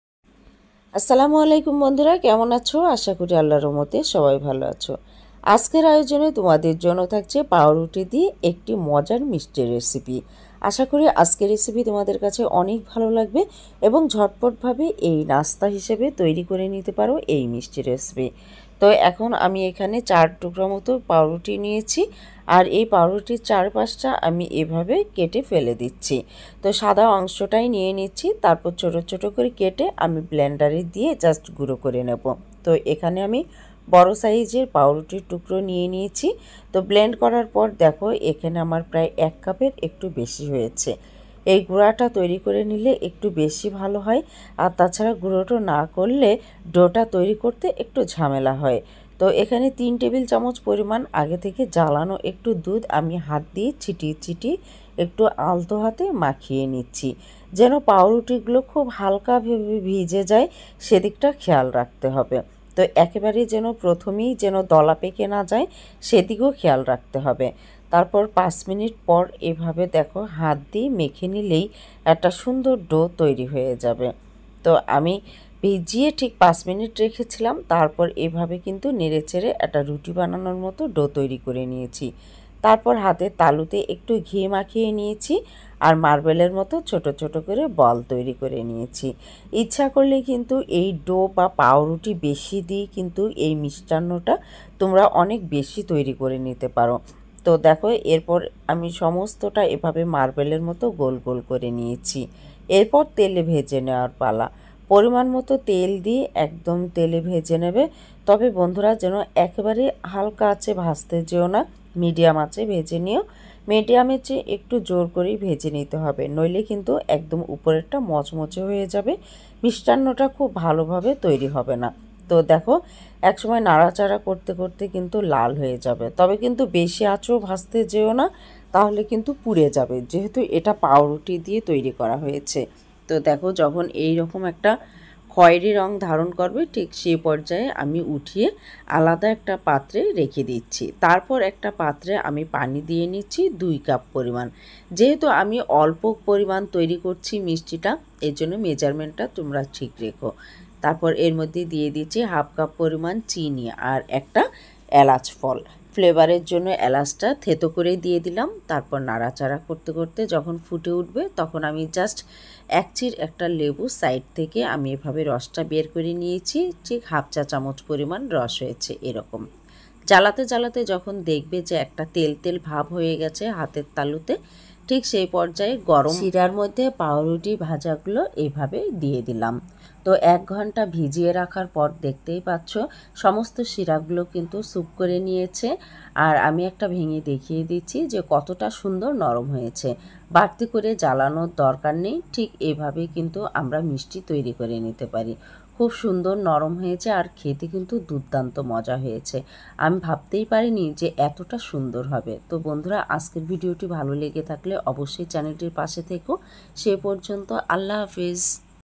৪ পিস পাউরুটি দিয়ে ঝটপট তৈরি করুন নরম তুলতুলে মজার মিষ্টি রেসিপি ll Bread Sweet Recipe
৪ পিস পাউরুটি দিয়ে ঝটপট তৈরি করুন নরম তুলতুলে মজার মিষ্টি রেসিপি ll Bread Sweet Recipe
Ingredients :
Bread pieces : 4 nos
Milk: 3 tbsp
Oil: As needed
Water : 2 Cup
Sugar : 1/2 Cup
Lemon juice: 1/2 tsp
Cardamom : 1 nos